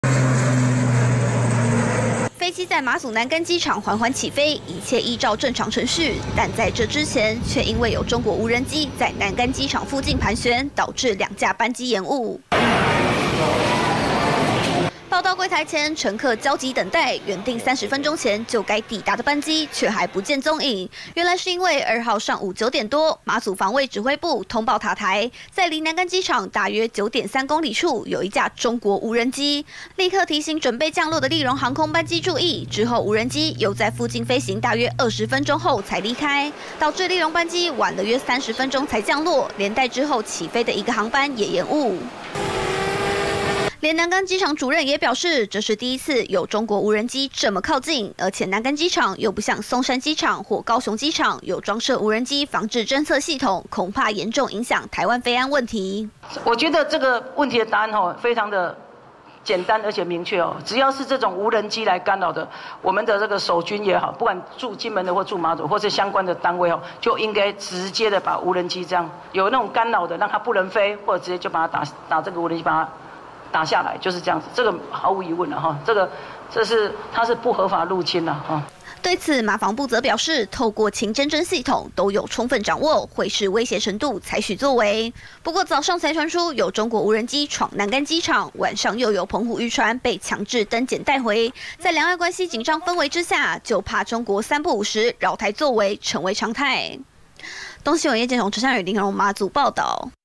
中國無人機來亂！擾馬祖南竿機場起降 2航班延誤@newsebc
馬祖南竿機場2號上午9點多，馬防部通報在距離機場大約9.3公里處，竟然出現中國無人機不斷來回飛行，導致立榮航空有2個航班因此延誤。連機場主任也說，這是第一次有中國無人機這麼靠近機場。在馬祖沒有無人機防制系統的情況下，恐怕成為飛安漏洞。但馬防部則表示，都在掌握之中。